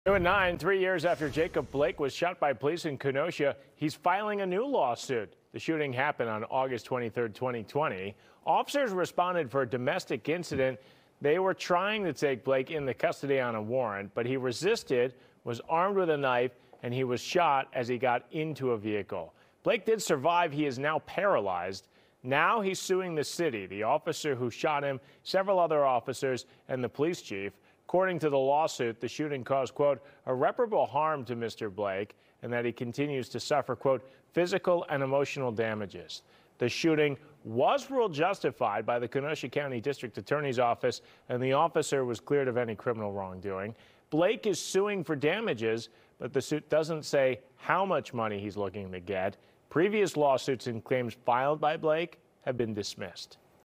Jacob Blake sues Kenosha, officers 3 years after shooting | FOX6 News Milwaukee
Jacob Blake has filed a new civil lawsuit against the city of Kenosha and police after he was shot by an officer in August 2020.